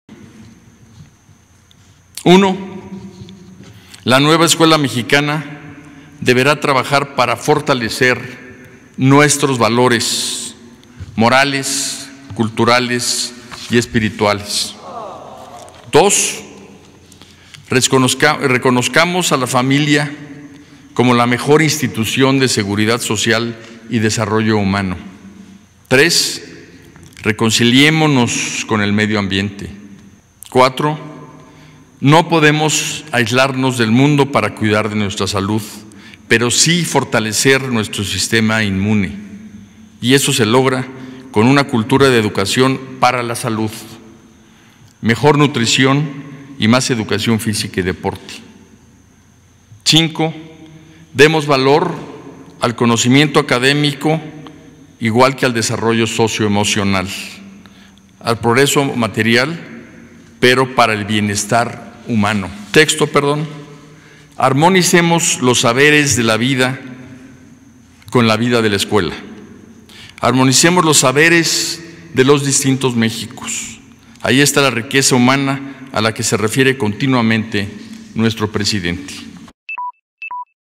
¿A dónde debemos encaminar el sistema educativo?
Esteban Moctezuma Barragan da 6 puntos para encaminar al sector educativo 🦆